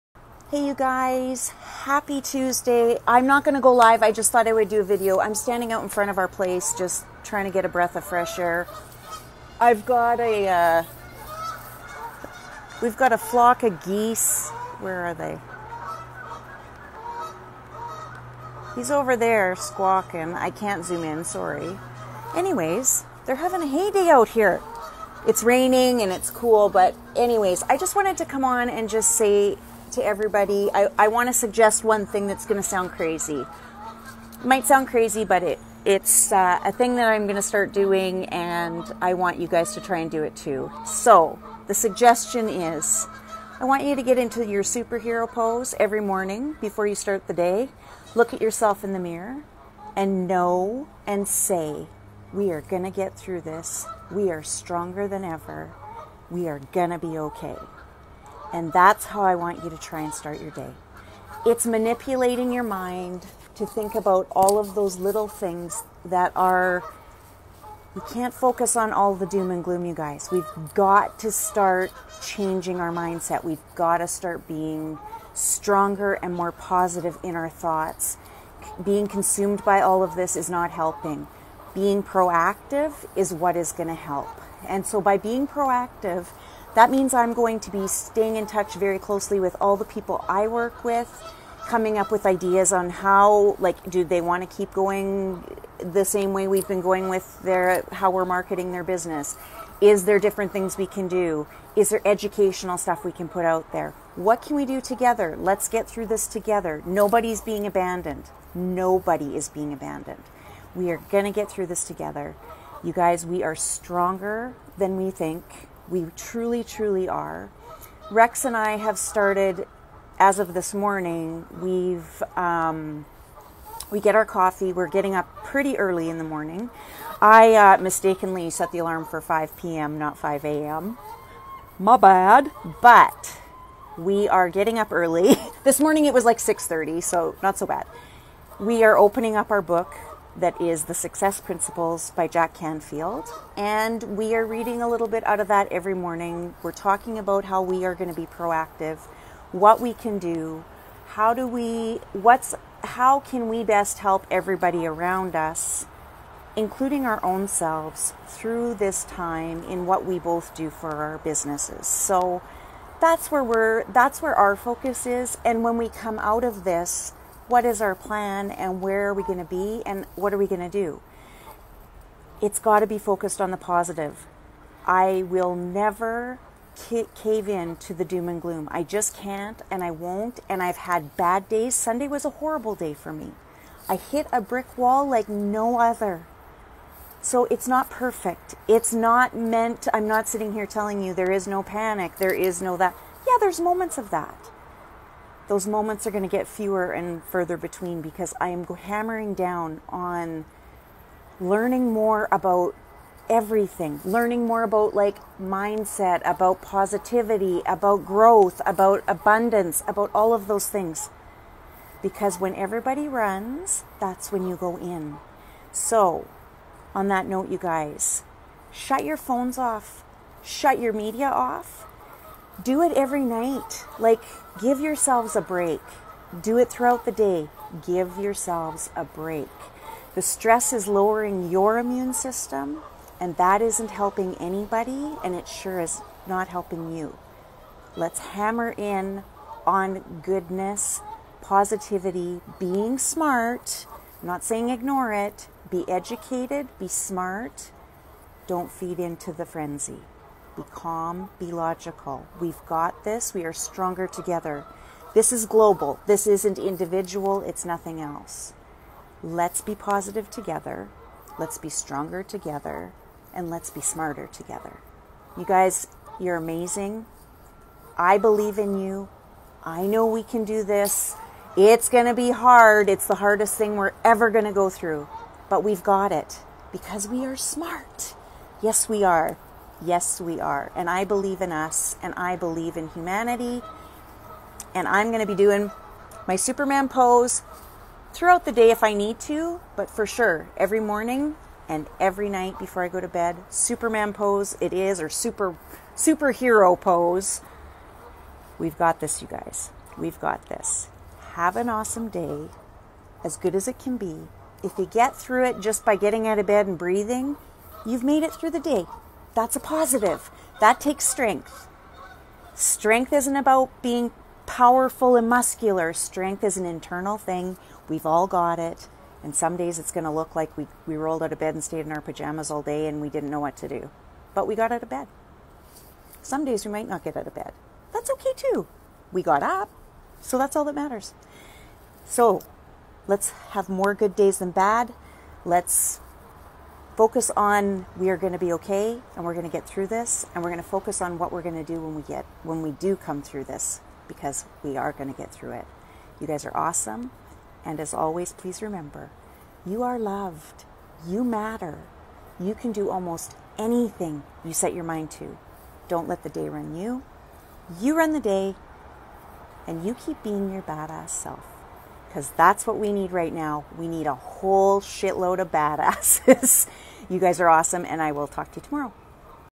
Super Hero Pose
Every single morning before you do anything I want you to get your super hero pose happening